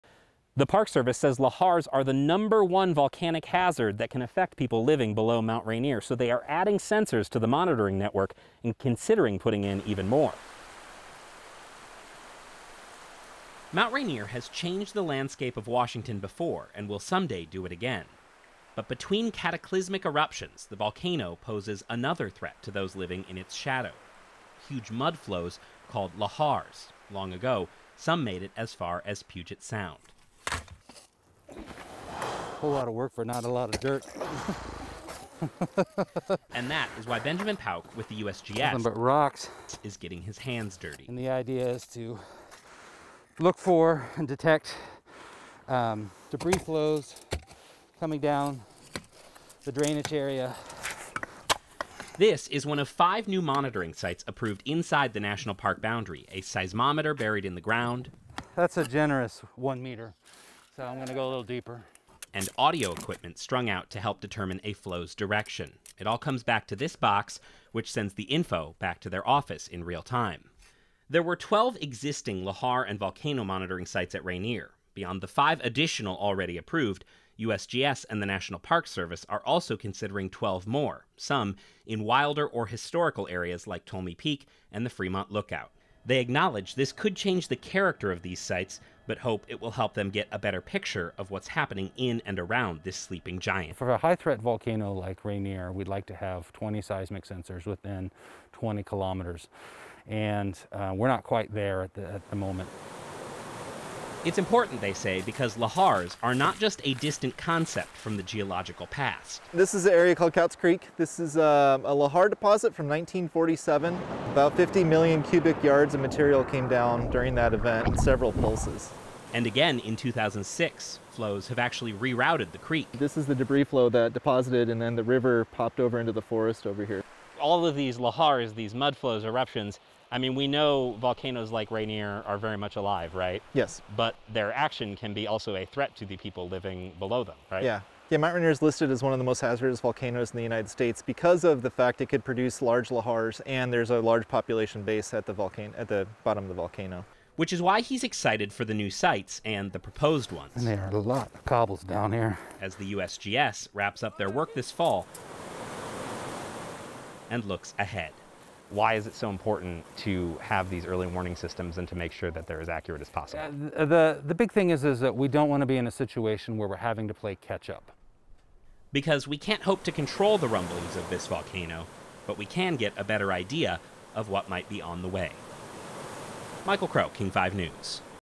Scientists adding more sensors to detect lahars on Mount Rainier
Lahars are huge mudflows that come down from a volcano, typically along a river valley, and can threaten those living nearby. Officials at Mount Rainier are working to get a better on handle on detecting Lahars.